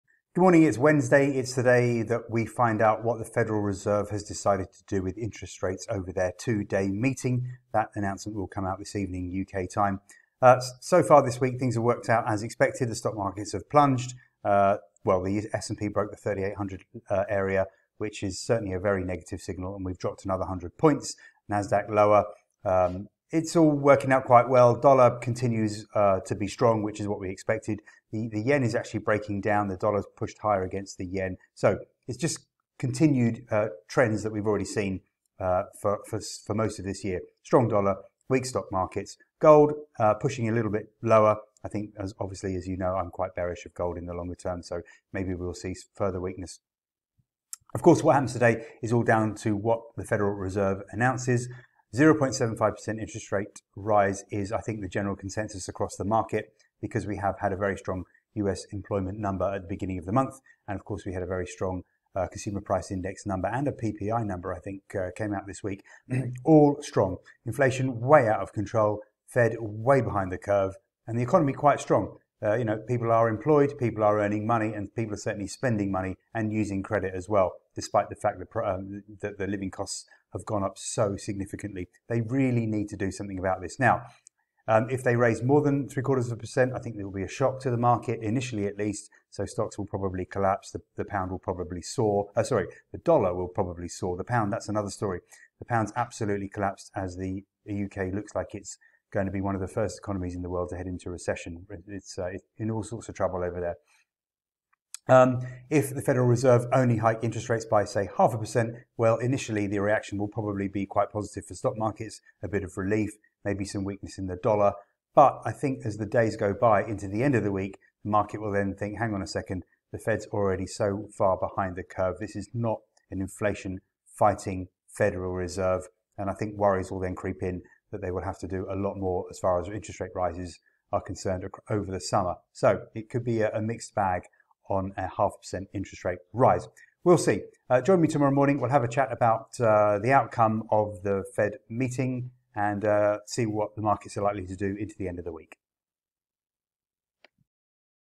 Federal Reserve meet: 0.5% or 0.75% interest rate rise in the US? How are markets expected to react?
The US Federal Reserve ends their 2 day meeting today & will announce their decision on how higher interest rates will rise from today. General expectations are for a 0.75% rise?
How will this affect the markets & what happens of they rise rates by only 0.5% after the strong employment, CPI & PPI data released earlier this month?

daytradeideas.co.uk